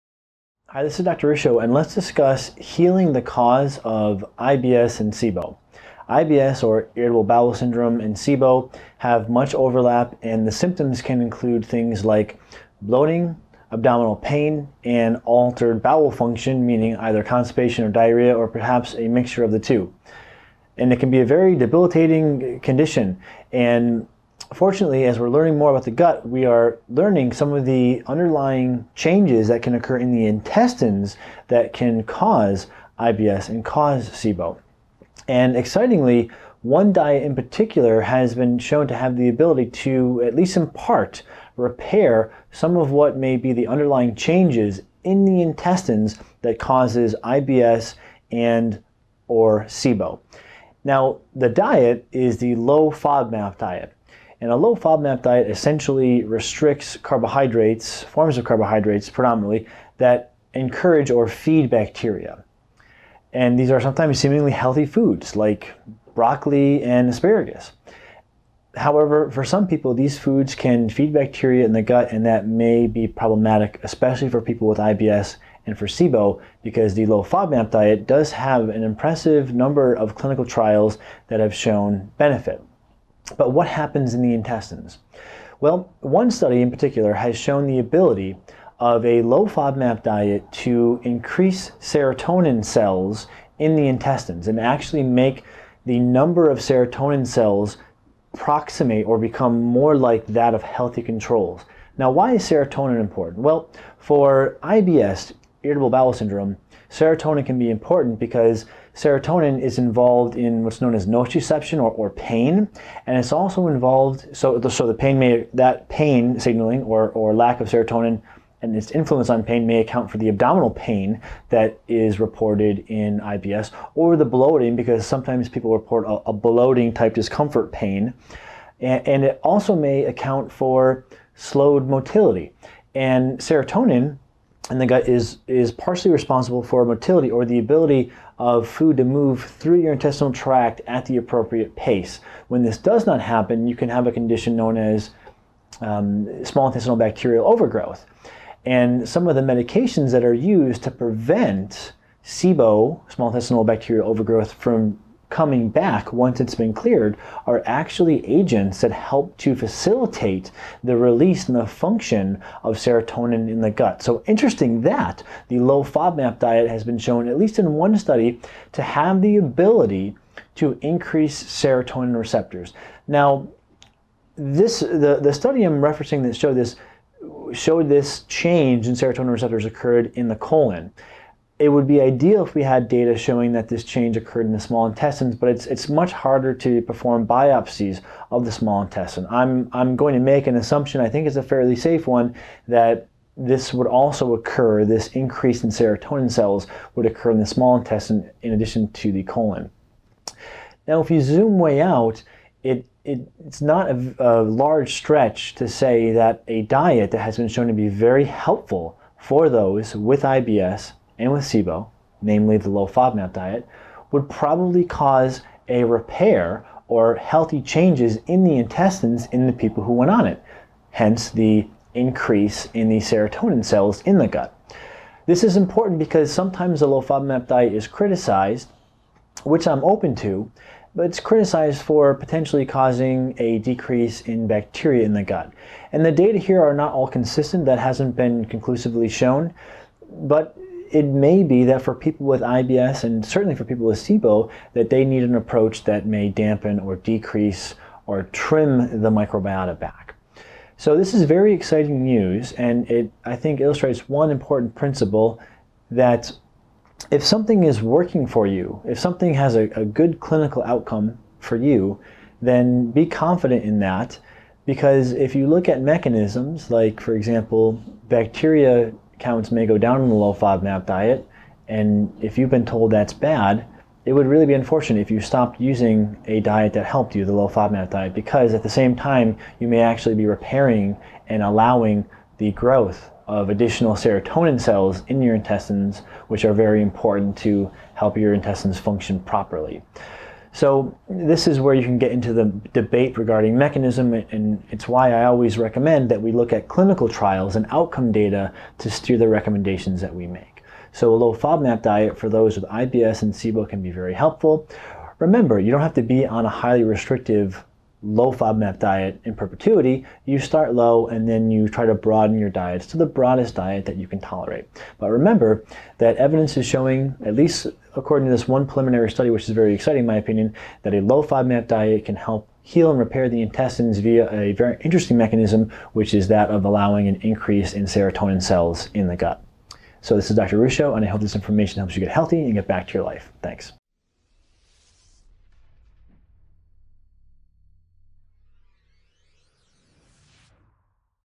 Addressing the Cause of IBS and SIBO
IBS and SIBO are common and can interfere with quality of life.  Symptoms include bloating, abdominal pain, and altered bowel function (constipation or diarrhea).  As we are learning more about IBS and SIBO, we are learning that alterations in the intestines may be a cause.  Excitingly, recent evidence suggests that certain dietary changes may help repair the intestines in those with IBS and SIBO.  Let’s discuss.  Want more info?